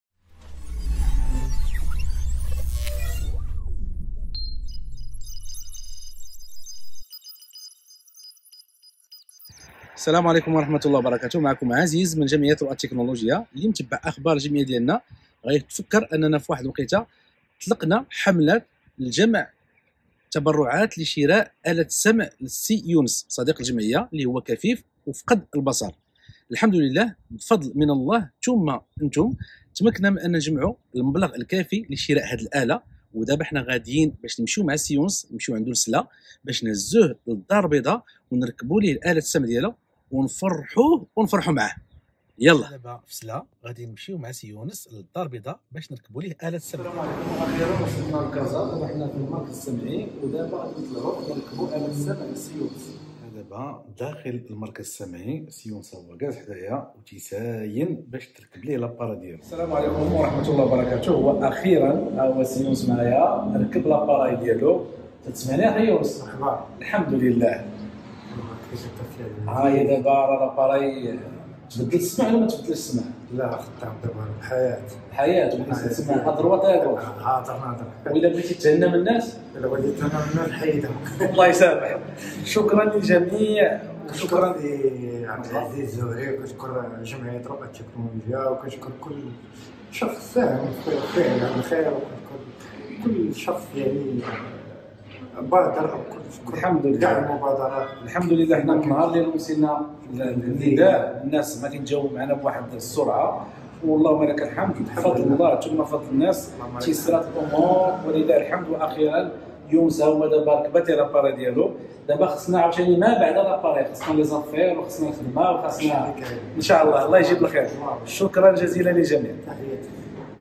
‏آلة السمع لكفيف الحمد لله
‏السلام عليكم
‏الحمد لله بفضل الله ثم فضلكم تمكني من شراء آلة السمع للصديق يونس كانت الفرحة لا توصف فز الله كل من شارك في هذا العمل الخيري
￼ تحياتي ￼￼￼